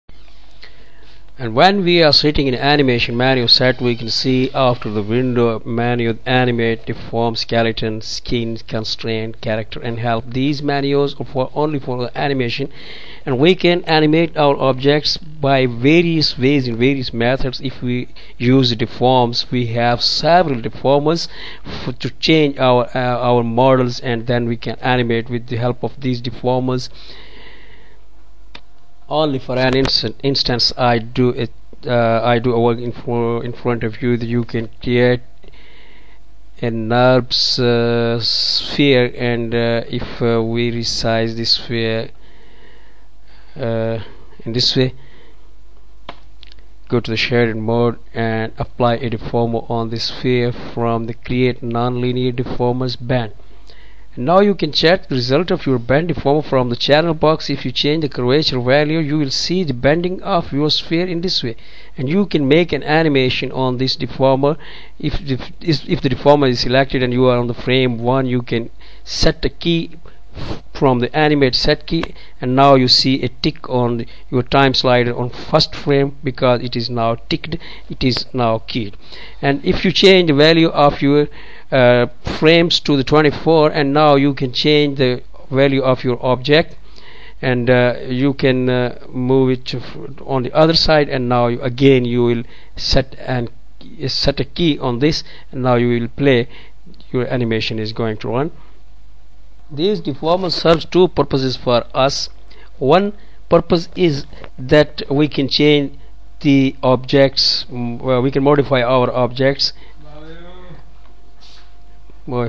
Animating Deformers with Maya
for more detailed free video on animating deformers. This simple video is prepared as a trailer of detailed video. You may find numerous free videos on animation if you are really interested in learning animation with Maya.